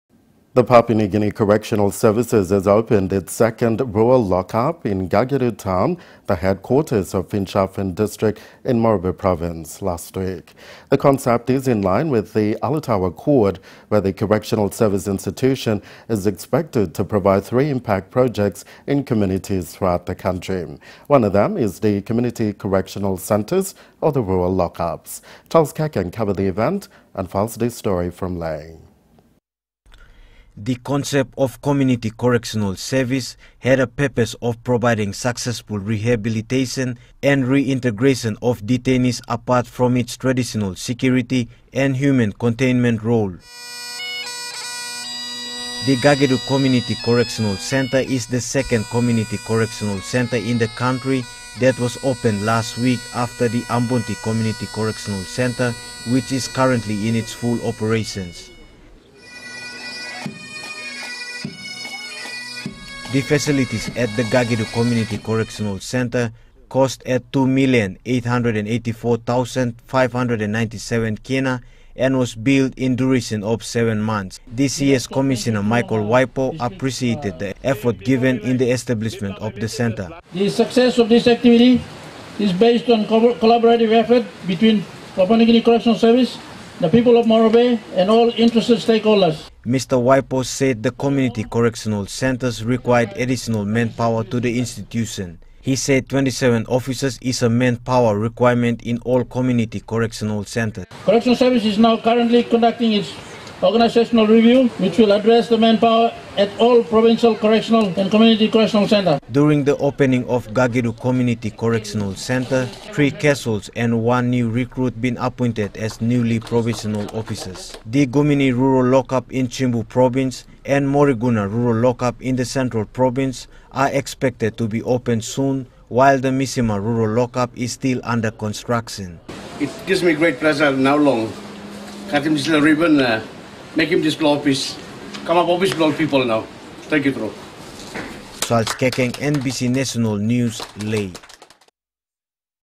NBC News - Rural lockup open
The Papua New Guinea Correctional Service has opened its 2nd Rural Lock Up in the Gagidu town, the haedquarters of Finchaffen District in Morobe Province last week